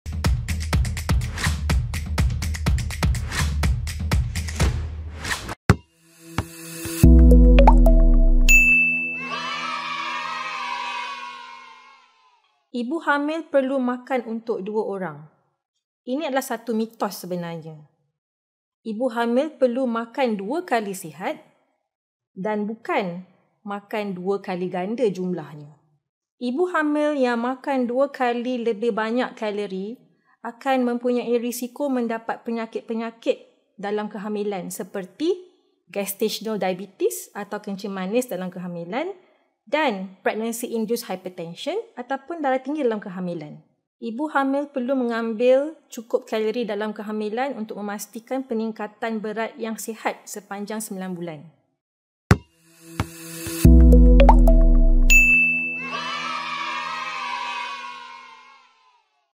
Can you eat for two? Do you need the extra calories?
🩺 Gain insights from expert Dr. Nurzarina Abdul Rahman on whether you need extra calories while eating for two, potential health issues related to overeating during pregnancy, and how it can impact your well-being.